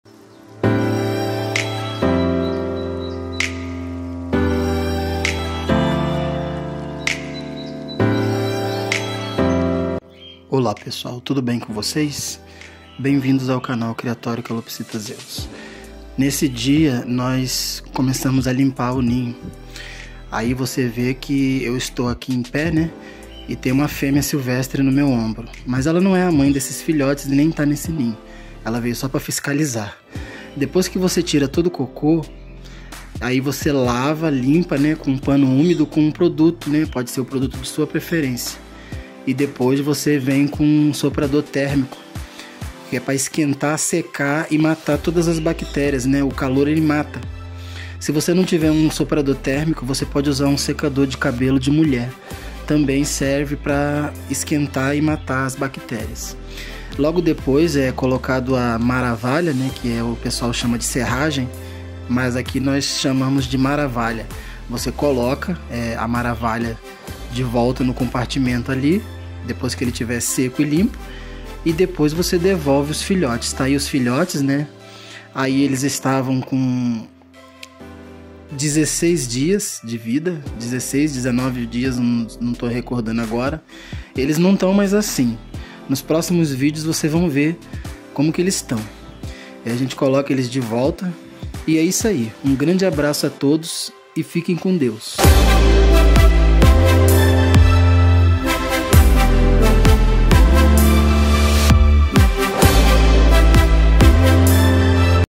Limpeza do Ninho de Calopsitas
Criatório Calopsita Zeus apresenta:

Limpeza do Ninho - Parte I - Outros Filhotes

Acompanhem neste vídeo a limpeza do ninho de calopsitas. Use máscara! Seja prudente!
Está apreciando o nosso perfil no Instagram? Siga-nos!

Abraço do Criatório Calopsita Zeus, o Seu Criatório.